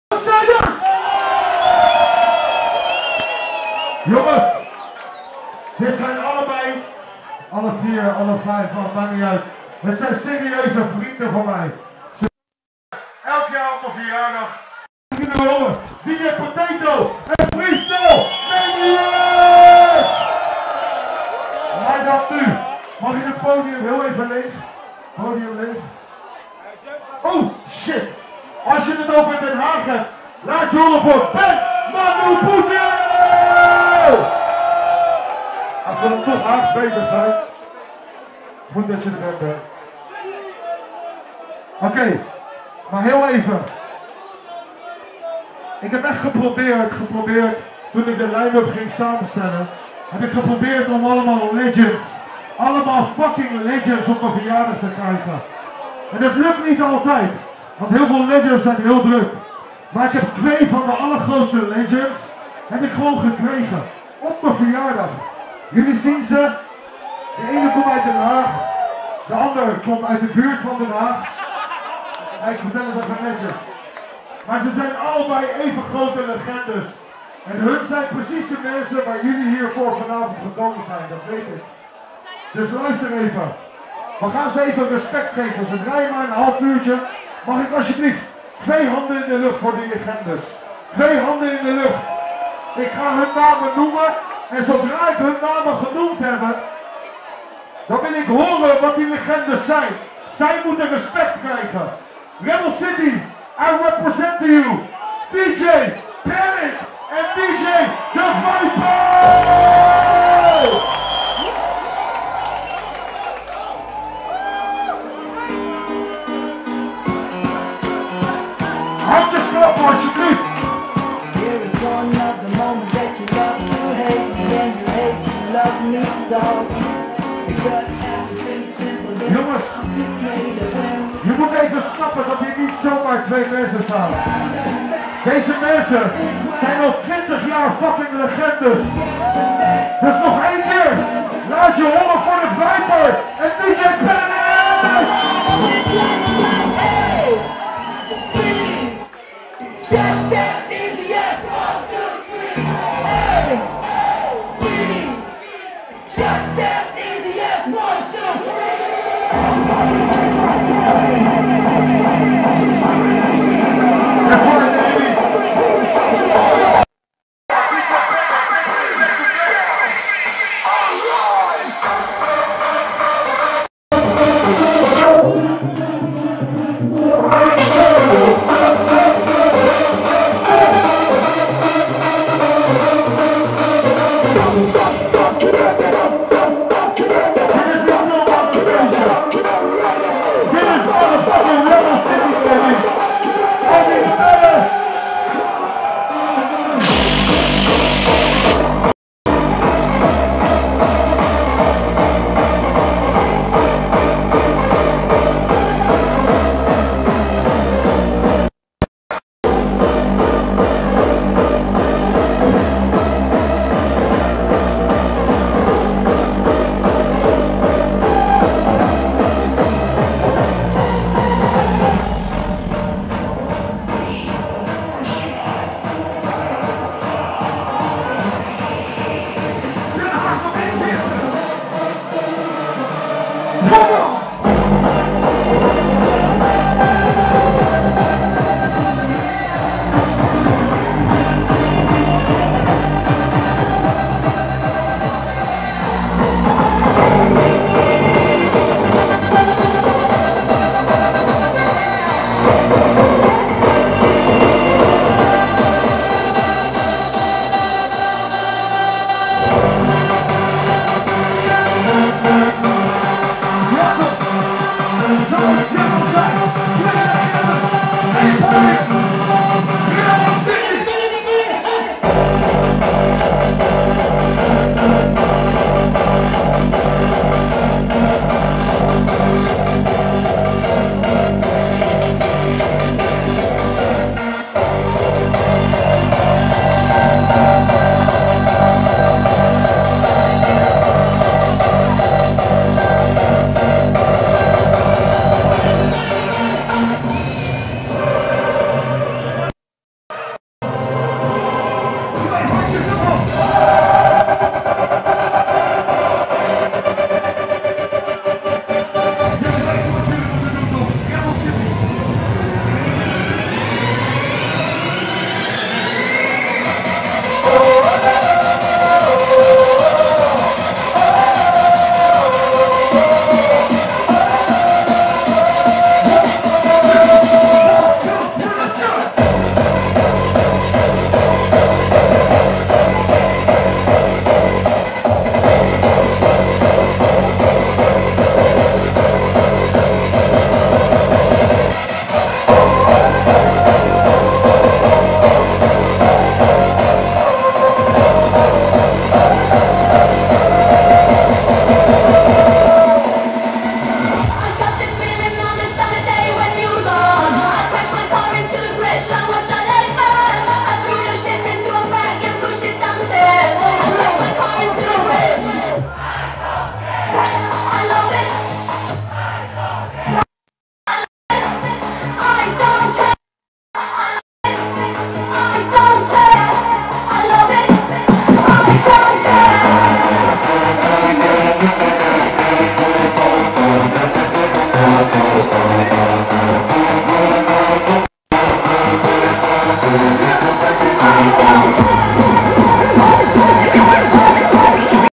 2017 Rebl city at the beach - 02-07-2017 - aankondiging Panic en Viper plus de eerste track
2017 Rebl city at the beach - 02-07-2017 - aankondiging Panic en Viper plus eerste track